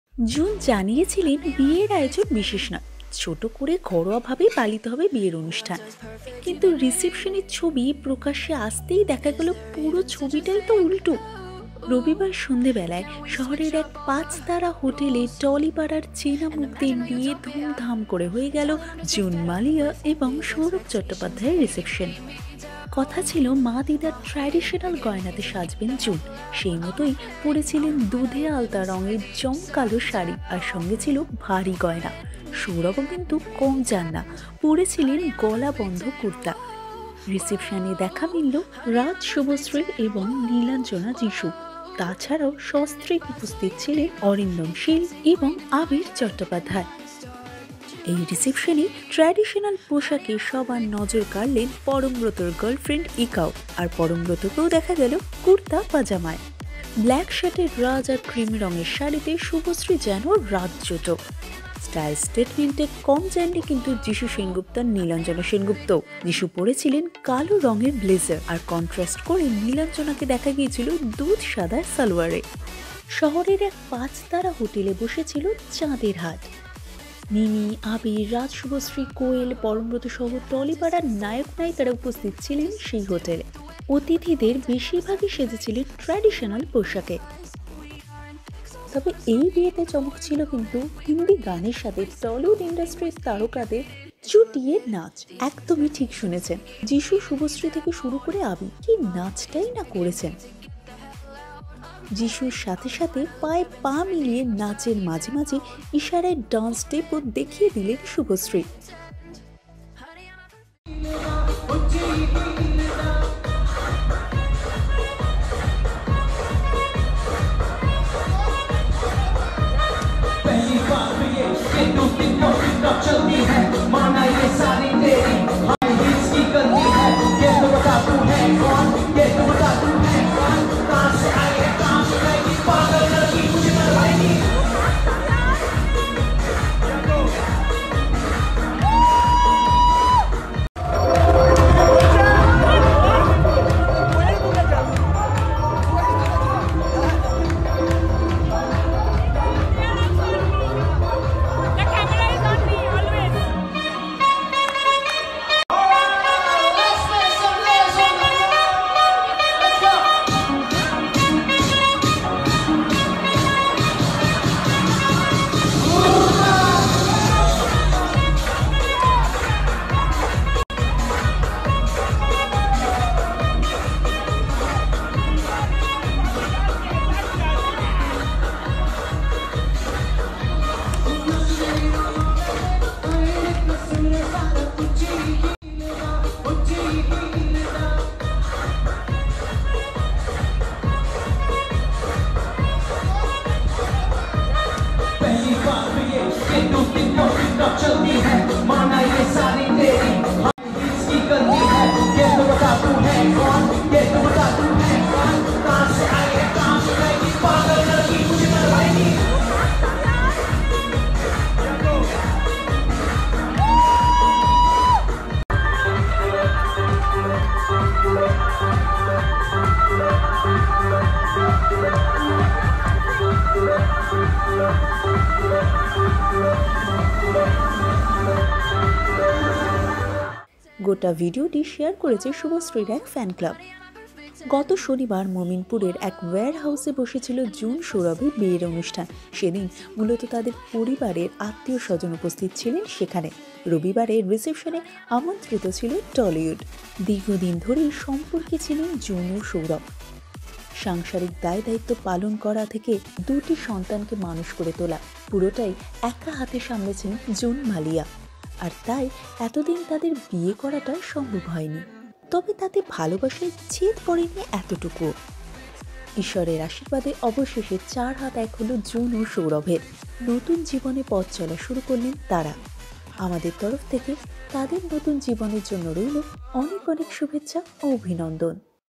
জুন মালিয়ার রিসেপশনে শুভশ্রী - যিশুর Party Dance | Subhashree | Jishu | Raj |VIRAL TOLLYWOOD
On the reception of JuneMalia and Sourav Chatterjee, SubhashreeGanguly & JishuSengupta shows their energy level of dance & enthusiasm.. Have a look on this..